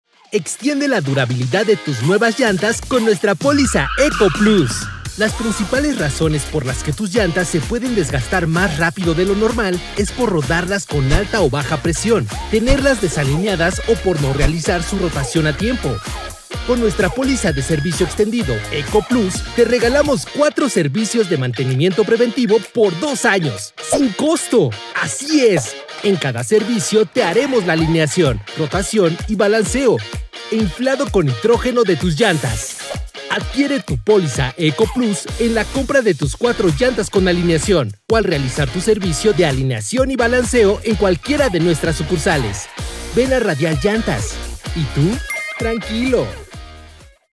Eco-Plus Servicio Extendido | Radial Llantas
Maximiza la vida útil de tus llantas haciendo los servicios de mantenimiento. Al comprar tus llantas en Radial, te regalamos nuestra Póliza Eco Plus, dándote 4 servicios de mantenimiento gratis, 1 por cada 6 meses. Incluye alineación, balanceo, rotación e inflado con nitrógeno. ¡Cuida tu inversión, y ahorra con Radial!